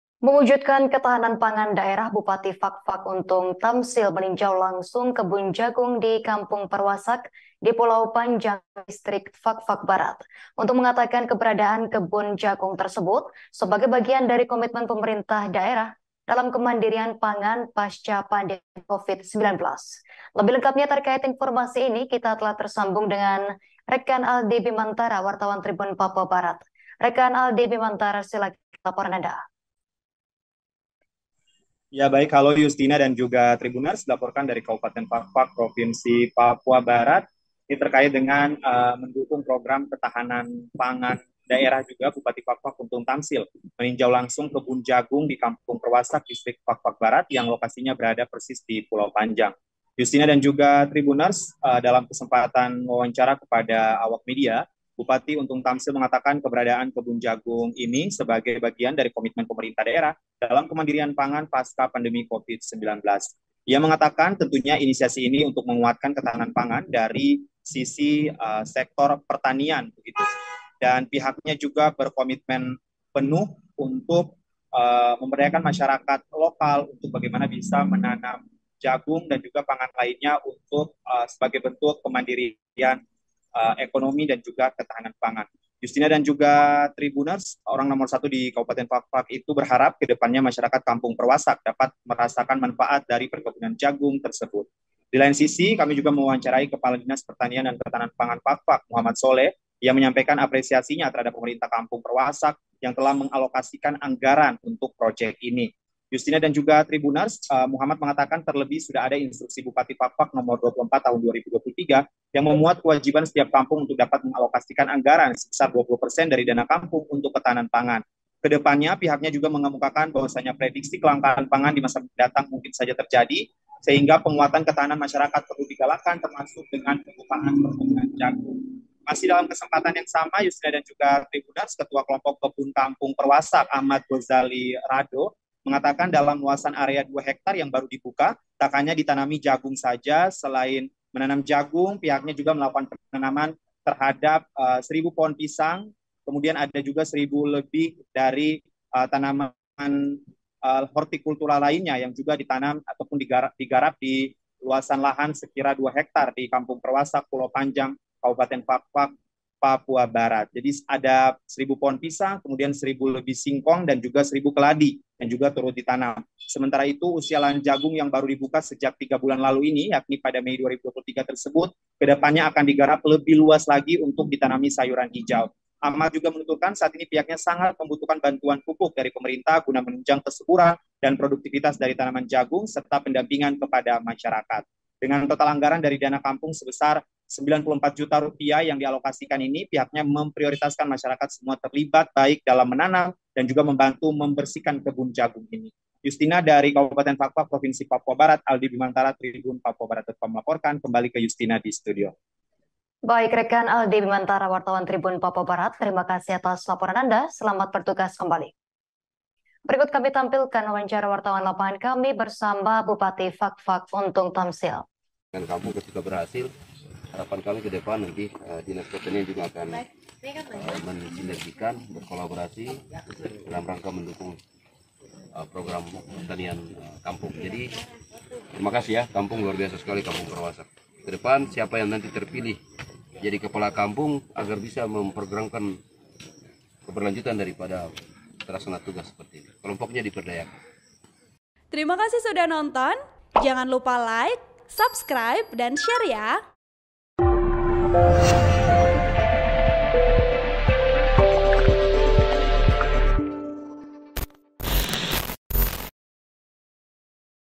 Bupati Fakfak Tinjau Kebun Jagung di Pulau Panjang: Inisiasi Menguatkan Ketahanan Pangan
Bupati Fakfak Komitmen Kuatkan Ketahanan Pangan, Tinjau Kebun Jagung di Pulau Panjang

TRIBUN-VIDEO.COM - Mewujudkan ketahanan pangan daerah, Bupati Fakfak, Untung Tamsil, meninjau langsung kebun jagung di Kampung Perwasak di Pulau Panjang, Distrik Fakfak Barat.

Untung mengatakan keberadaan kebun jagung tersebut sebagai bagian dari komitmen pemerintah daerah dalam kemandirian pangan pasca-pandemi Covid-19.(*)

Tribun Papua Barat-Aldi Bimantara

VP: Salim Maulana